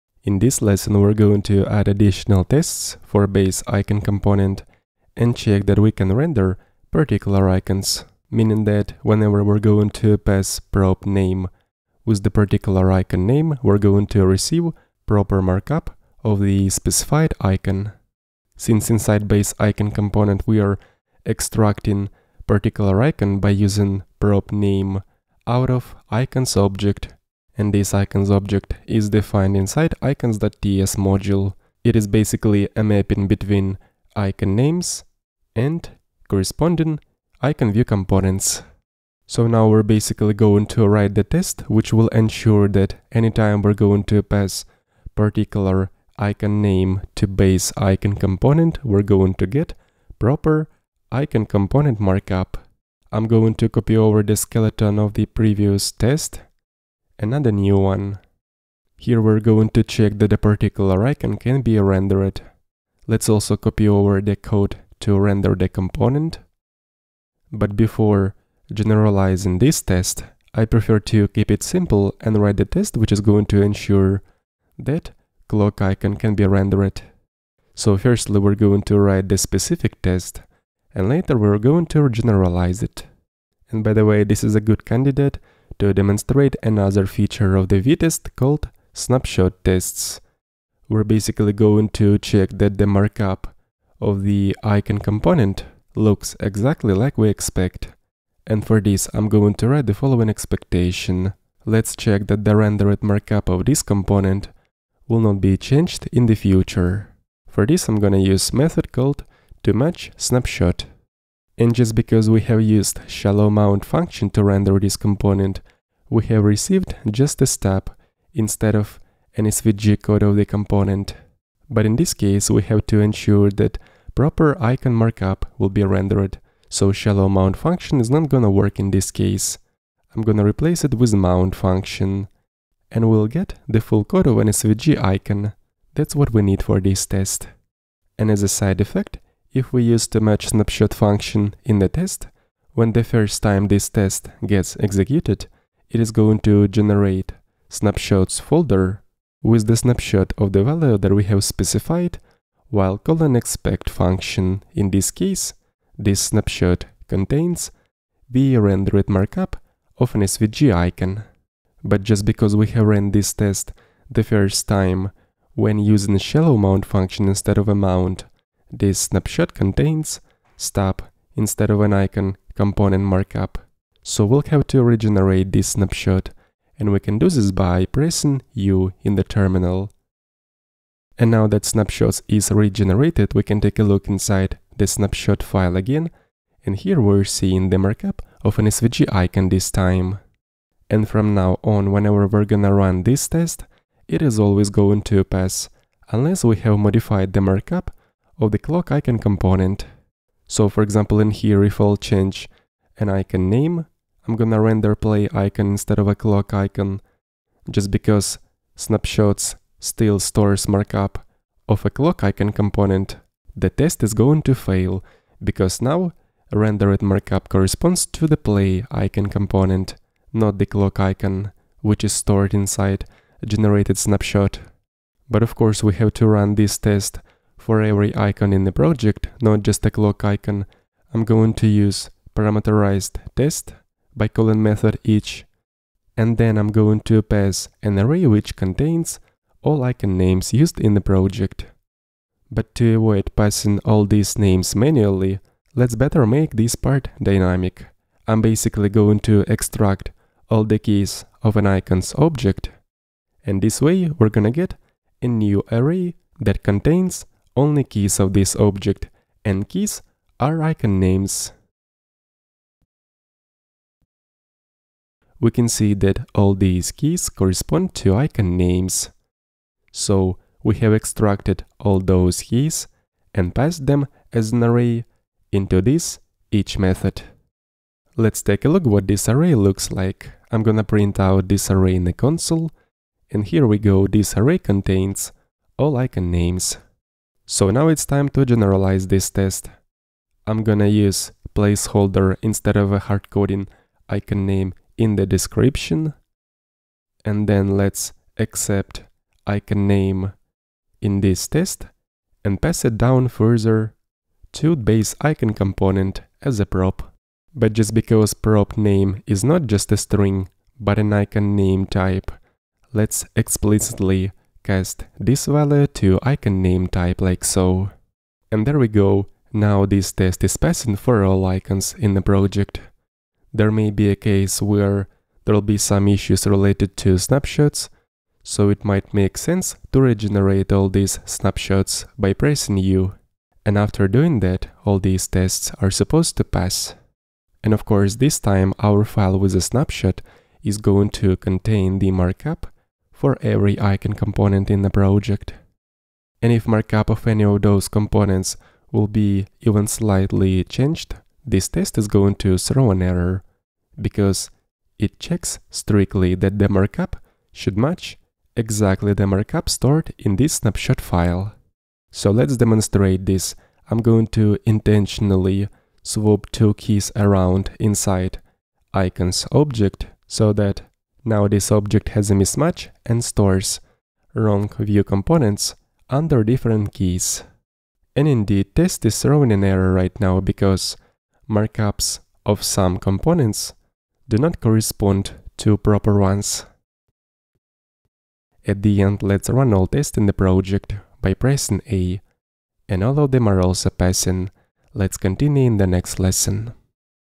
Vue Unit Testing #6 - Icon component test (snapshots)
Unit testing of Vue application by using testing framework Vitest and Vue Test Utils library.